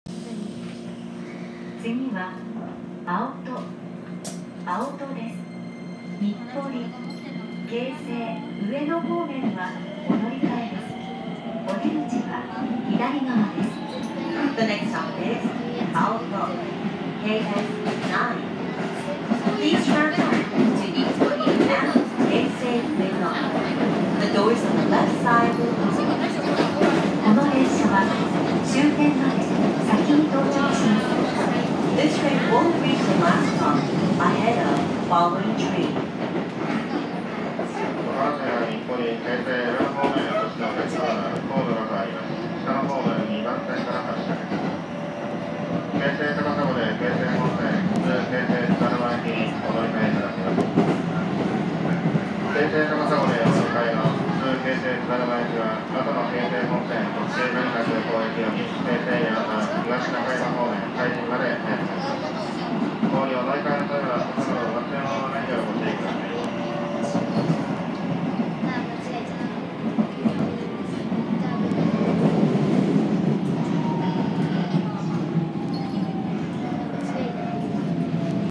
京成押上線 普通印旛日本医大行き 青砥駅到着前車内放送
京成押上線普通印旛日本医大行きの青砥駅到着前の車内放送です。